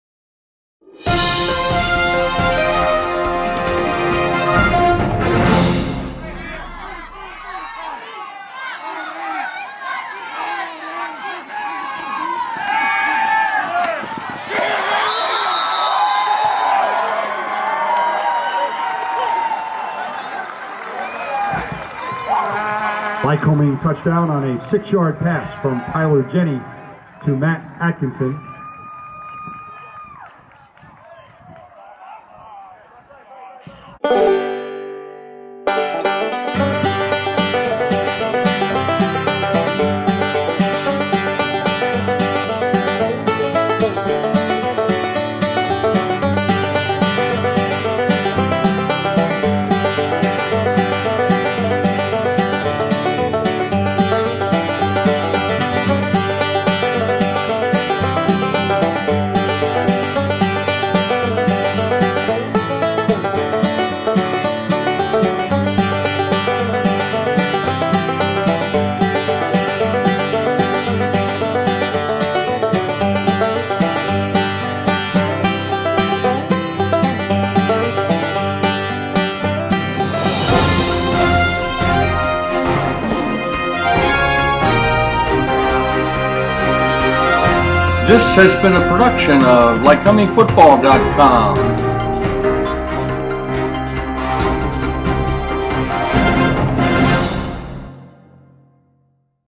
2013 Lycoming Football - Another Jenny to Matt Atkinson Touchdown Vs Stevenson
Tyler Jenny completes 67% of his passes and on this touchdown he threads the needle between two Stevenson defenders to sure handed WR Matt Atkinson. For sure, Atkinson will go down Lycoming and MAC football as one of the Legendary wide receivers. Great year Matt. PS: Notice RB Craig Needhammer picking up the ball and giving it to the Ref's. This is Lycoming!